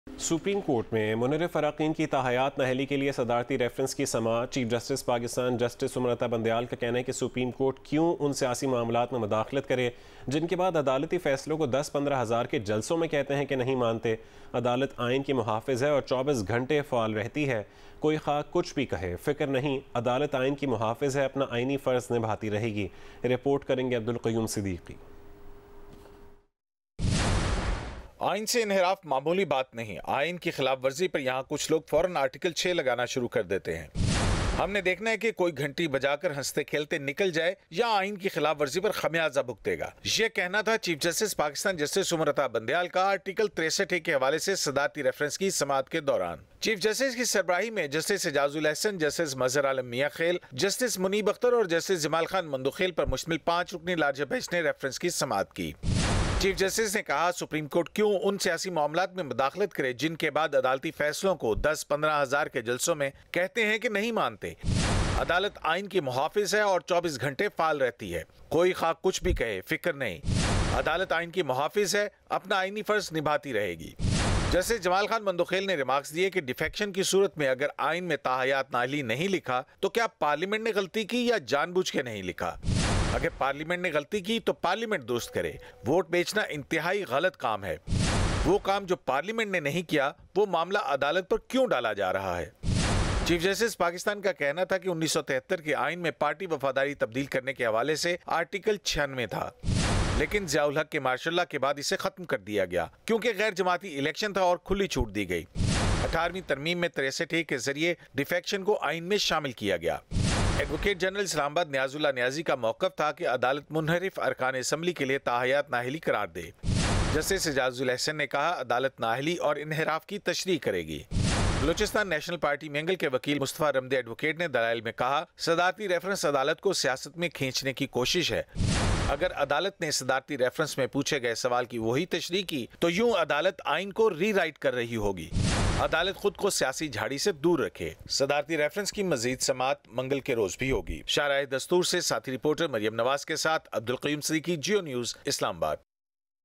Courts work for 24 hours, CJP says after Imran Khan's criticism | Supreme Court of Pakistan | PTI
Courts work for 24 hours, CJP says after Imran Khan's criticism
ISLAMABAD: Courts work for 24 hours, Chief Justice of Pakistan (CJP) Justice Umar Ata Bandial said Monday in response to former prime minister Imran Khan's question on why the courts had to open at midnight on April 9 — the night he was ousted as the prime minister.
No one needs to point a finger at the courts, CJP Bandial said during the hearing on a presidential reference seeking interpretation of Article 63A of the Constitution of Pakistan.

The top judge said: "We don't care about what is being said on social media; [we] are the protector of the Constitution".

A five-member larger bench of the Supreme Court — headed by Justice Bandial and comprising Justice Muneeb Akhtar, Justice Aijazul Ahsan, Justice Mazhar Alam, and Justice Jamal Khan Mandokhel — had resumed hearing the reference on Monday.

During the hearing, the CJP Bandial said that the SC expects political leaders to defend judicial orders in public.

The remark came in response to former prime minister Imran Khan's recent criticism of the courts.

The apex court was also hearing PTI Chairman and ex-premier Imran Khan's plea seeking to declare dissident party members ineligible for life.

PTI's lawyer Babar Awan requested the court issue notices to the respondents of the case.

At this, CJP Bandial asked Awan to present his case first, saying that the notices will be issued afterwards.

During the hearing, the Islamabad Advocate-General Niaz Ullah Khan Niazi contended that lifelong ineligibility should be awarded over defection.

He asked the court to rule early, keeping the situation in Punjab and Centre in view.

"Parliament hasn't included lifelong disqualification in Article 63A," Justice Mandokhel remarked, asking if Parliament deliberately avoided including the lifelong disqualification in Article 63A or by mistake.

Justice Mandokhel noted that half of the people would comply with the apex court's order and half won't as some of them favour lifelong disqualification and some are against it.

"Why do you want the SC to decide this when there is Parliament?" he asked.

At this, CJP Bandial remarked that his fellow judge's stance is clear.

"Why should we announce the verdicts when 10-15 thousand people start criticising judicial orders [...] why should the court take part in your political debates."

"We expect political leaders to defend judicial orders in public," CJP Bandial remarked, adding that protecting and ensuring the rule of law is the court's responsibility.

In his arguments, Balochistan National Party-Mengal’s counsel Mustafa Ramday said that the president, in his reference, asked the court to rewrite the Constitution.

The government did nothing for the “audio and video” issue mentioned in the presidential reference, he added.

At this, the PTI’s lawyer said that two of their lawmakers had approached the ECP over the matter.

Ramday said that the Centre kept silent over the regime change in Balochistan, adding "you have seen whatever happened in Islamabad and Punjab."
'Constitution wants to end the culture of defection'

During the hearing, the CJP remarked: “It is our responsibility to uphold and protect the Constitution,” and declared that deviation from the Constitution was a violation.

The CJP said that the Constitution wants to end the culture of defection in politics. The top judge said that they have to see the results of constitutional violations and asked whether the violators of the Constitution be allowed to go unpunished or if they should be held accountable.